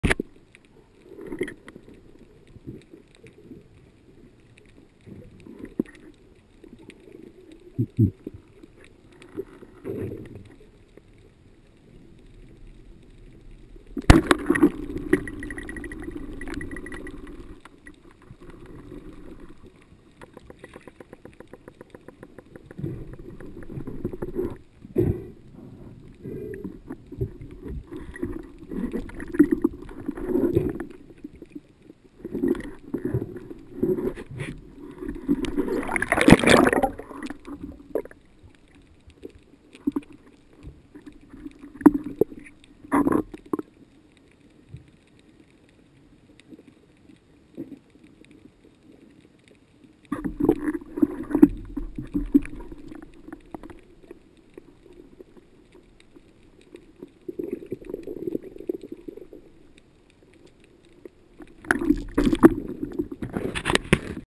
COPPIOLA DI LIZA AURATA (o cefalo dorato)..
esco un po tardi e mi accorgo di non essere stato il primo a passare: infatti trovo un deserto.. ma poi... ecco che con un tiro porto a casa il pranzo. Grazie a mia moglie che mi ha mandato di sabato a pesca. dimenticavo.. PRUDENZA SEMPRE!